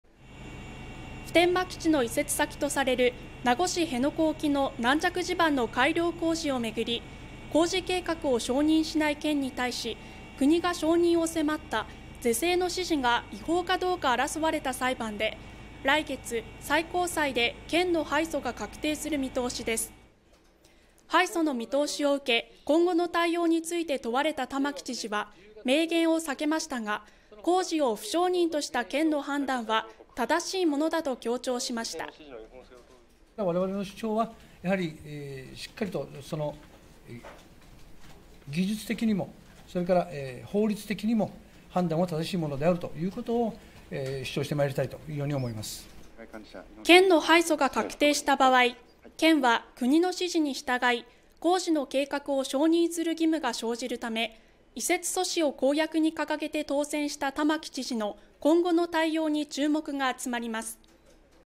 辺野古裁判敗訴で“承認の義務”が生じる玉城知事 辺野古移設阻止を公約に掲げ当選も 今後の対応に注目集まる
普天間基地の移設先とされる名護市辺野古沖の軟弱地盤の改良工事をめぐり、工事計画を承認しない県に対し国が承認を迫った『是正の指示』が違法かどうか争われた裁判で、来月、最高裁で県の敗訴が確定する見通しです。2023/08/25